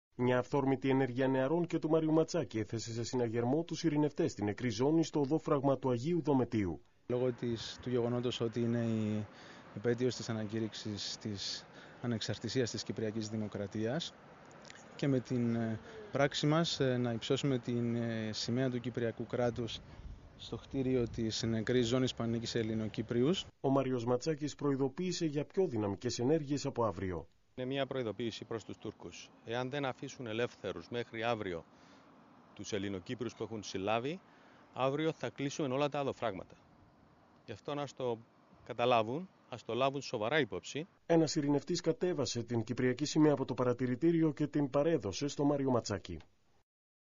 Stavros Hadjisavvas Οδόφραγμα Αγίου Δομετίου 1/10/2012
Σταύρος Χατζησάββας στη Νεκρή Ζώνη στο οδόφραγμα του Αγίου Δομετίου 1/10/2012 μαζί με τους Μάριο Ματσάκη και Ανδρέα Μασούρα. Ύψωση της σημαίας της Κυπριακής Δημοκρατίας ως η ελάχιστη συμπαράσταση προς τους 3 αστυνομικούς και 2 ψαράδες που παράνομα συνέλαβε το τουρκικό στρατιωτικό καθεστώς. ΤΕΡΜΑ ΣΤΗΝ ΠΟΛΙΤΙΚΗ του καναπέ! ΒΕΤΟ ΤΩΡΑ στην ενταξιακή πορεία της Τουρκίας!